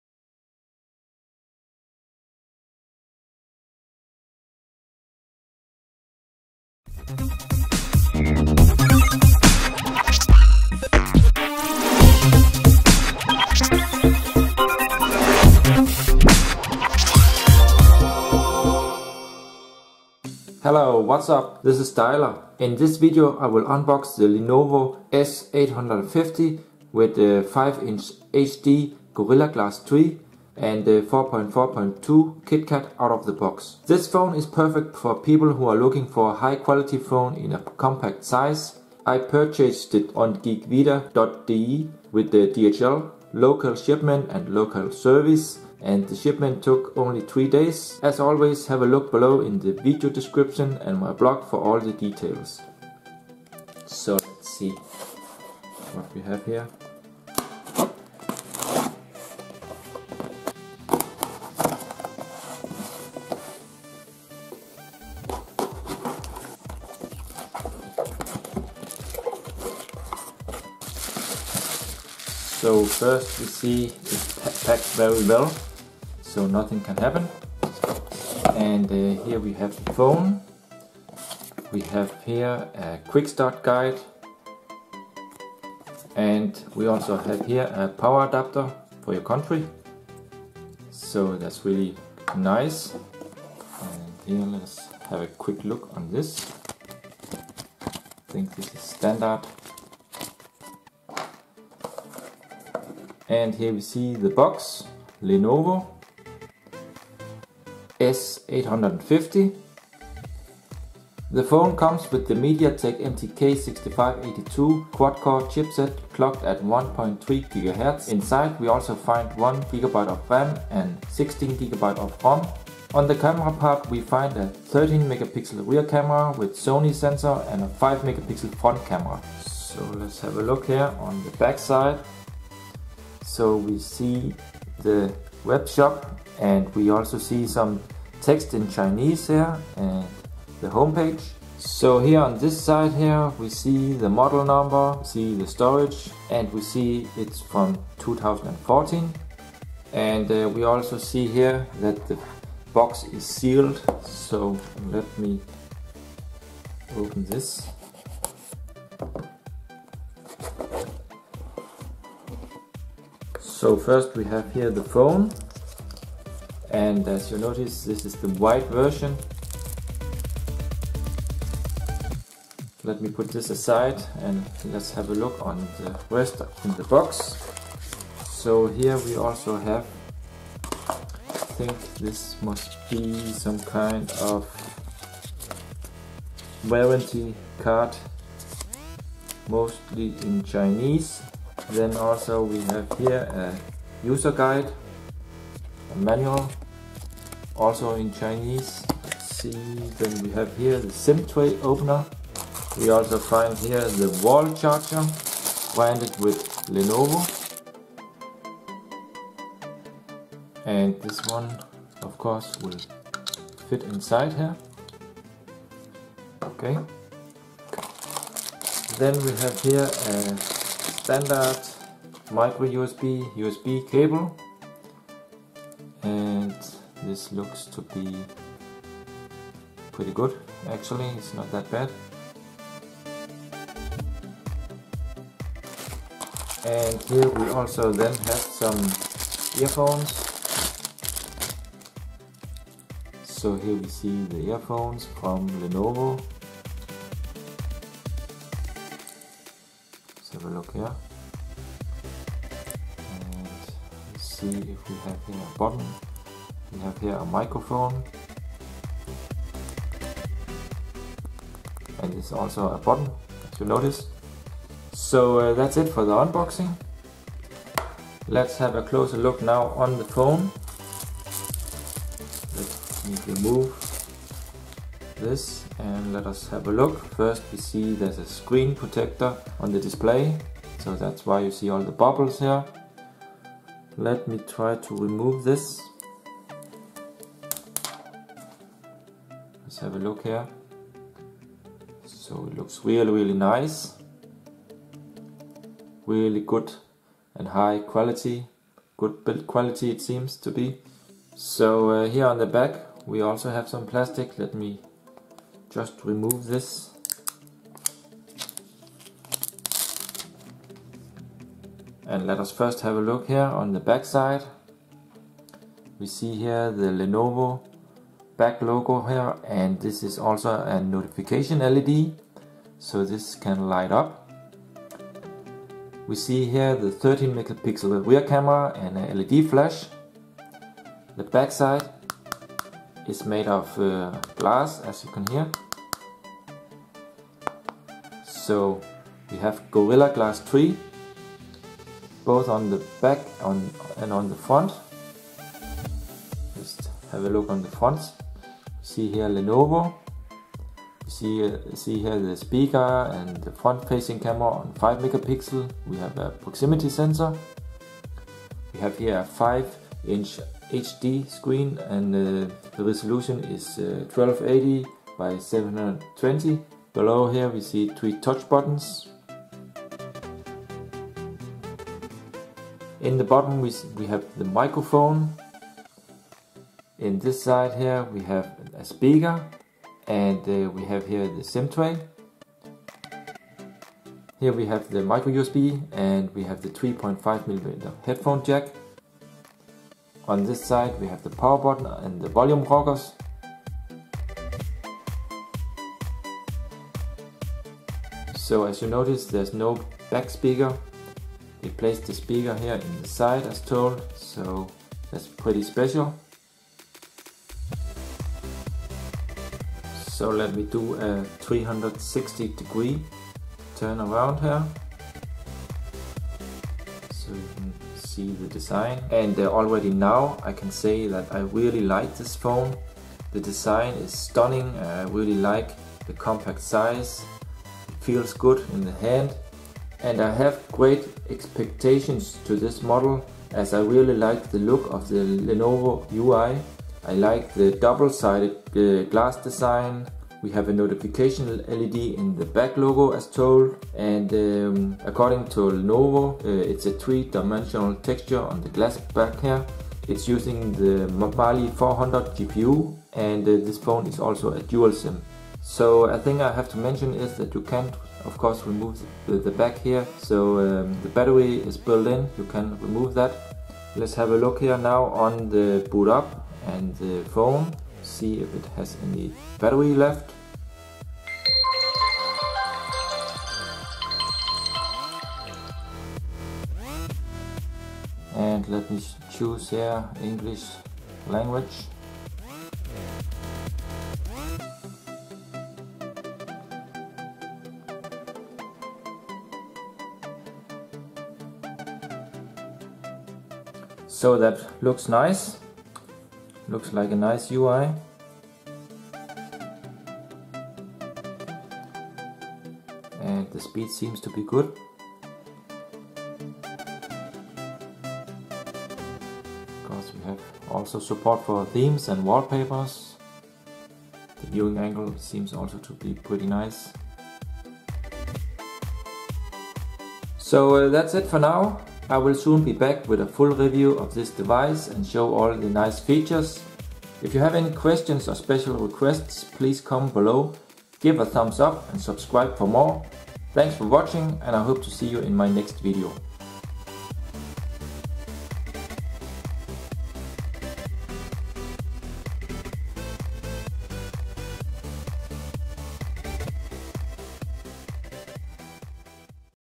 Lenovo S850 [UNBOXING] 5" HD IPS Gorilla Glass 3, Double Sided Glass, Notification Logo, MTK6582
Hightlights:
- Double-sided glass (front and back)
- Speaker is placed on the side/frame (not on the back)
- Dual SIM (Micro-SIM)
- 5.0 Inch HD 1280x720 Pixels
- 1 GB RAM / 16 GB ROM
- 13 MP backcamera with Sony sensor, 5 MP front camera
- Android OS, v4.4.2 (KitKat)
- Multilanguage
- Mediatek MT6582 Quad-core 1.3 GHz Cortex-A7
- GPU: Mali-400MP2
- Non-removable Li-Po 2000 mAh battery
- Notification LED light in the logo on the back